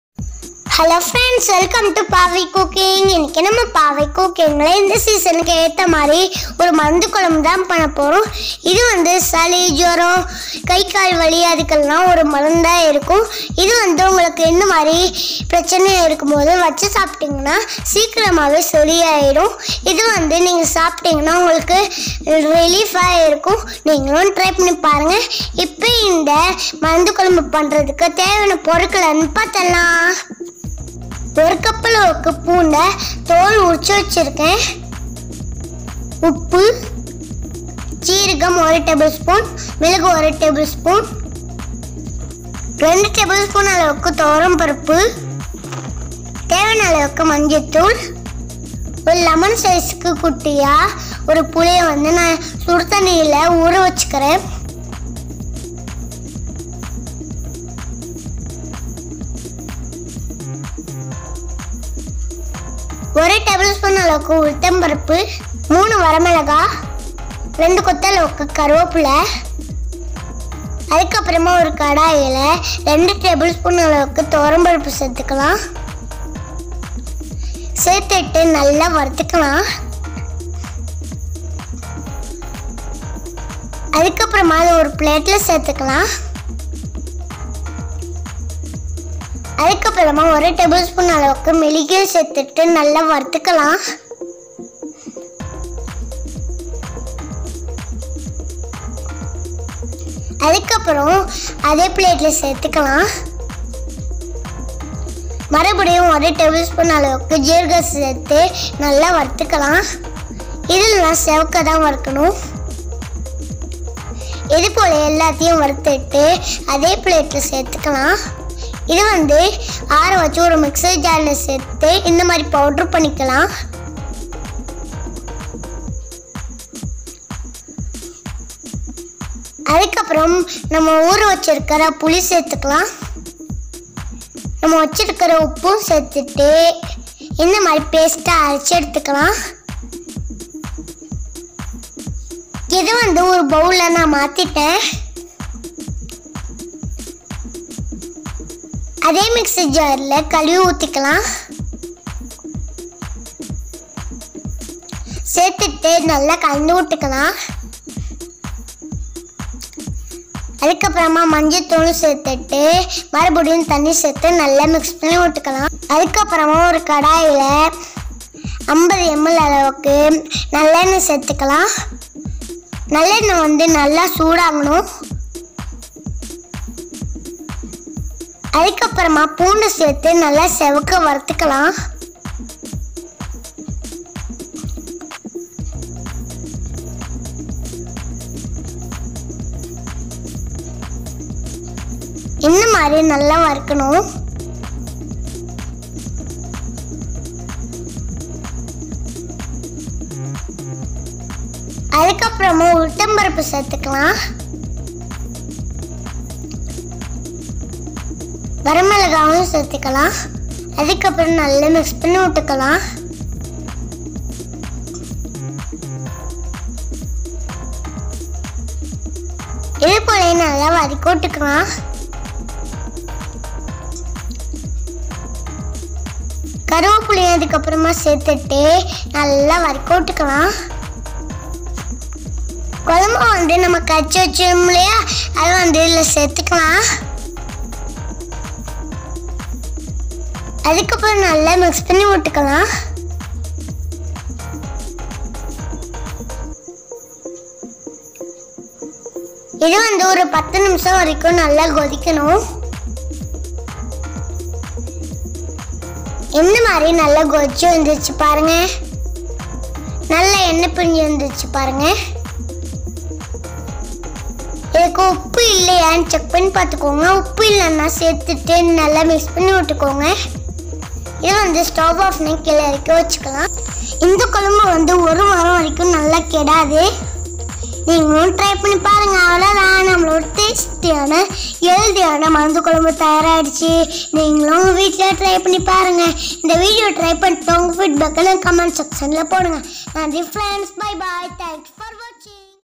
மருந்து குழம்பு செய்வது எப்படி/Kara Kulambu/Marundhu Kulambu /Poondu Kulambu Seivathu Eppadi Tamil
தேவையான பொருள்கள்/Ingredients:
பூண்டு/Garlic 1 cup
மிளகு/Pepper 1 tbsp
சீரகம்/jeera 1 tbsp
துவரம் பருப்பு/toor dal 3 tbsp
புளிக்கரைசல்/tarmaind lemon size
மஞ்சள் தூள்/turmeric powder ¼ tsp
நல்லெண்ணெய்/gingelly oil 100 ml
உளுந்து/ urad dal 1 tbsp
காய்ந்த மிளகாய்/ red chilli 2
கருவேப்பிலை/ curry leaves